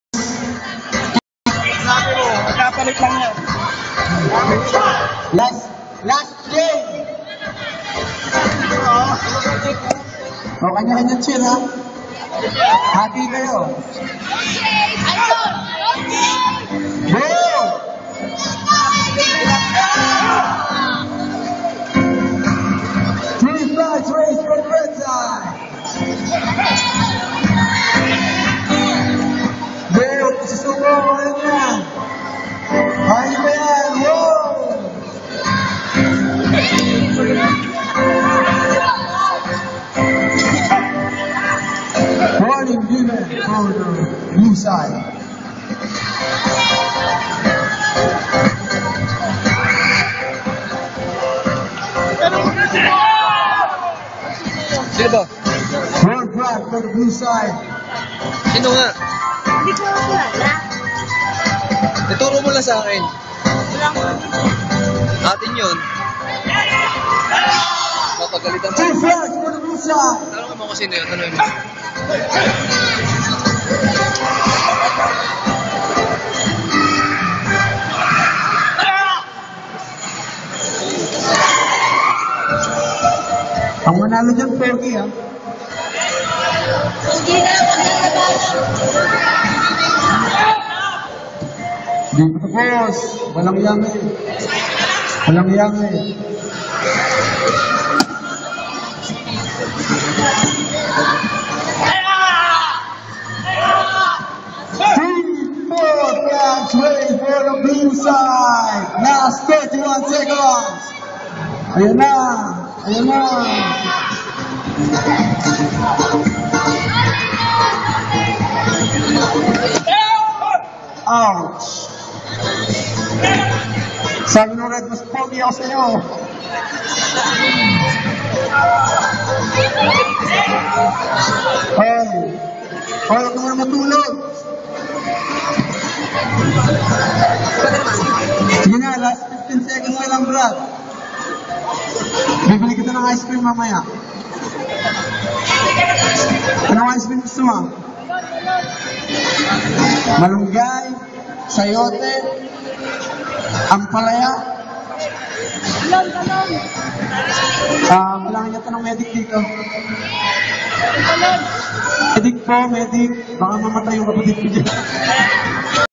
3rd AKKR Karatedo Championship - October 12, 2014
Championship game of my brother BLUE SIDE

3rd AKKR Karatedo Championships 2014
RED: Jayson Alerta 3
BLUE: David Varias 4